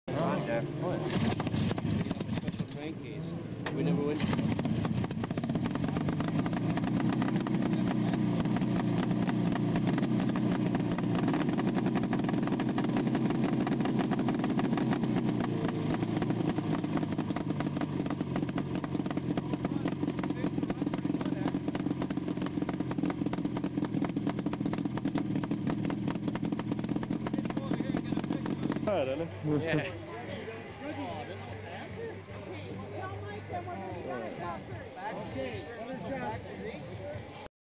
1967 Arctic Cat Panther factory racer
1967 Arctic Cat Panther factory racer at the Jake Woeckner memorial meeting Waupaca, Wisconsin in 1998.